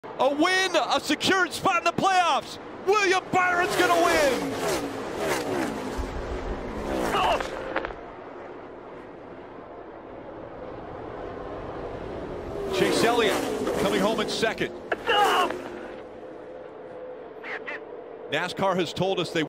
William Byron team communications have a worse stroke than Rick Allen himself.
He sounds worse than rick